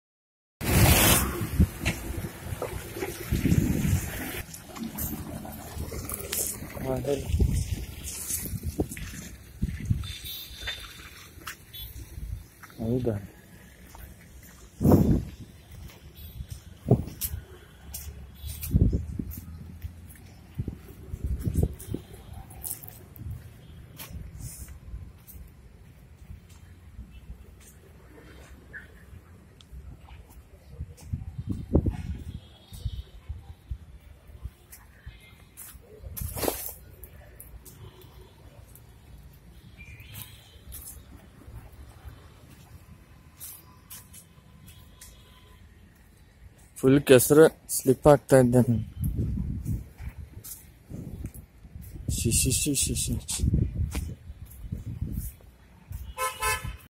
Walkthrough with UBL WDG4 twins in WDG4D dress | Belagavi station | Rainy morning
Date: 28/06/2019 13:30
Loco: UBL WDG4(repainted to WDG4D livery) 12111+12122